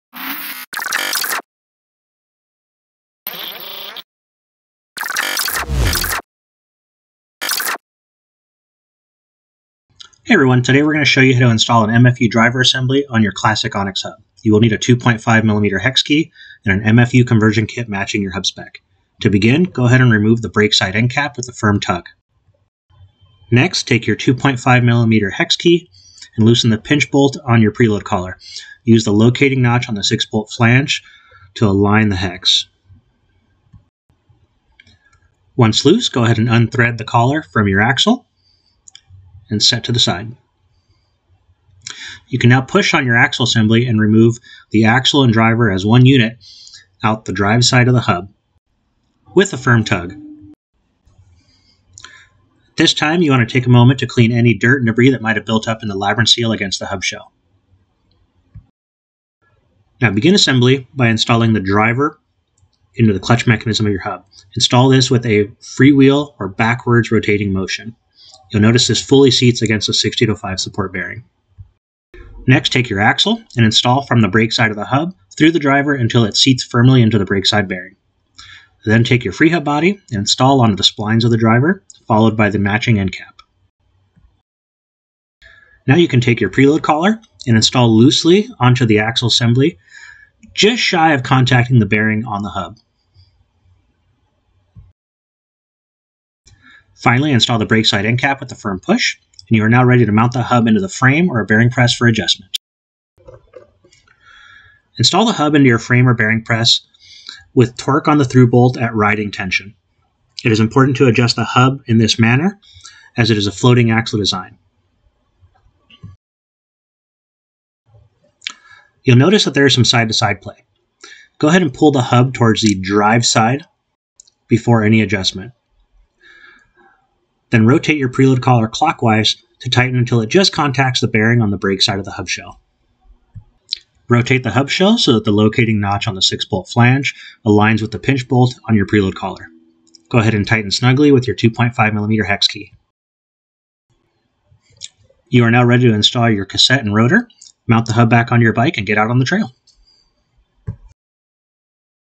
MFU Install on Classic ISO (6 bolt)
Guide to installing MFU driver assembly onto Onyx Classic ISO (6 bolt) hubs.